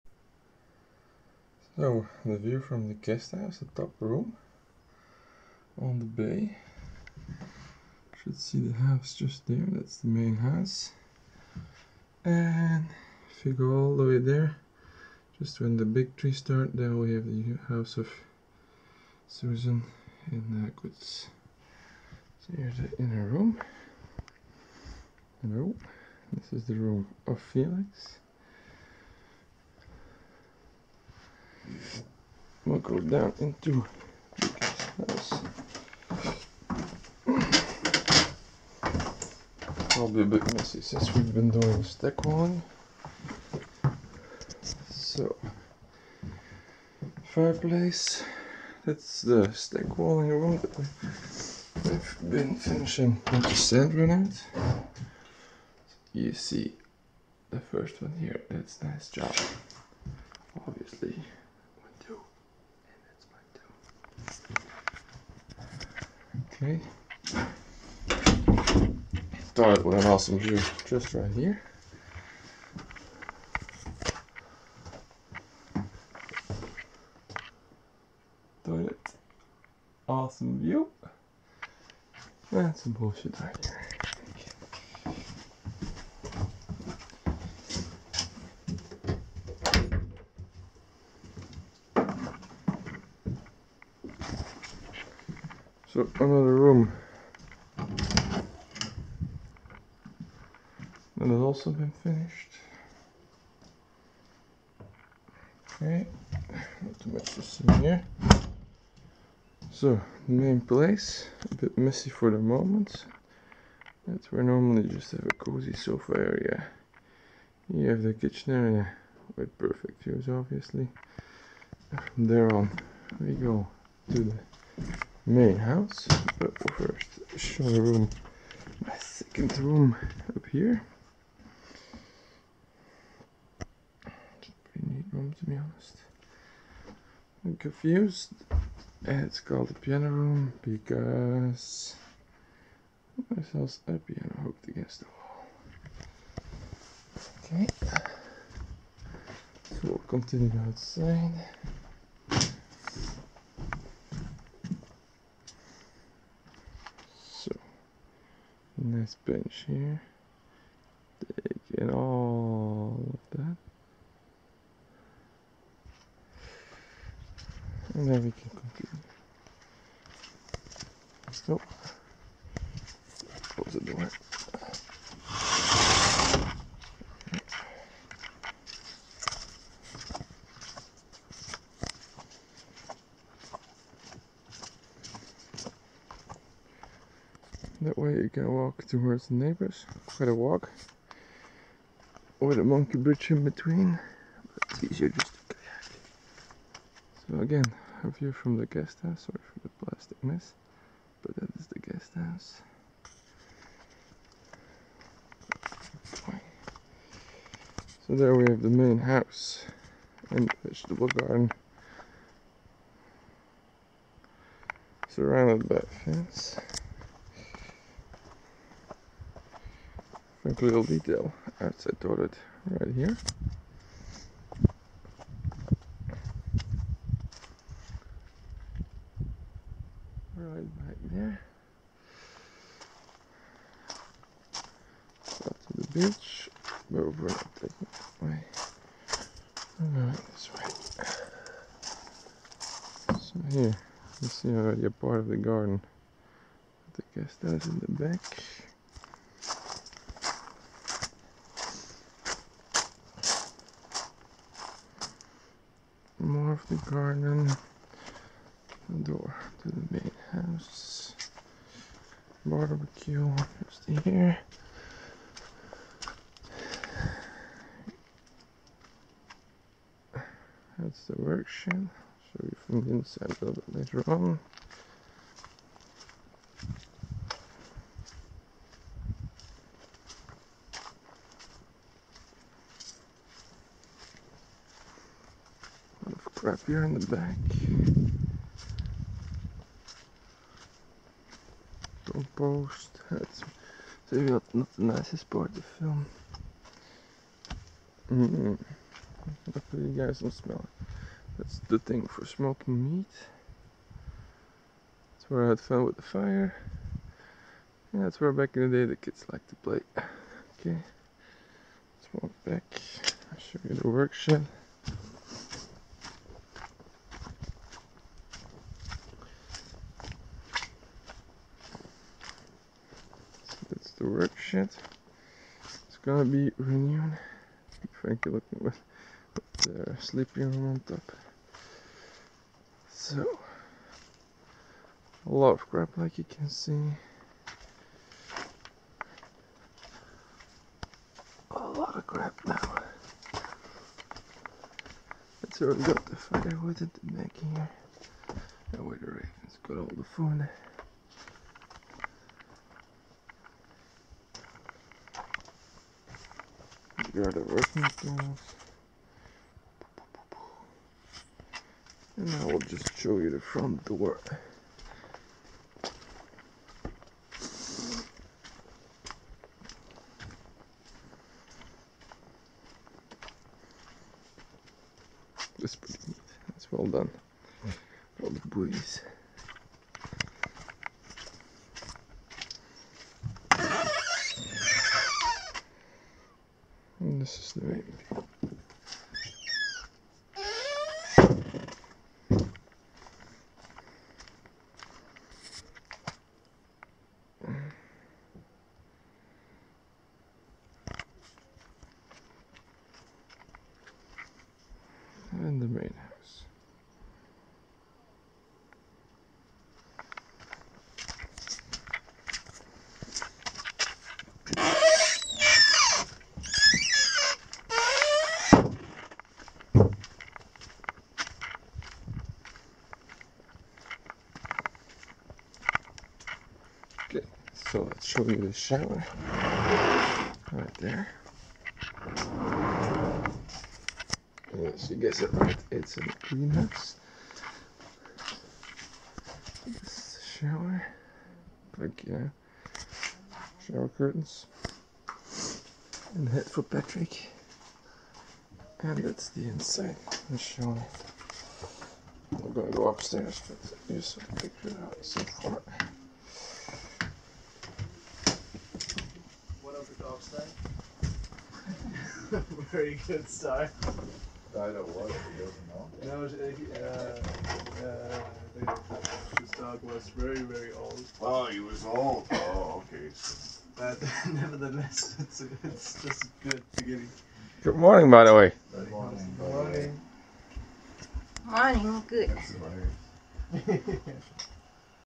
Rose Harbour Overview
A quick walk-through of Gwaii Haanas guesthouse, where I stayed during the fall of 2015 for about 6 weeks.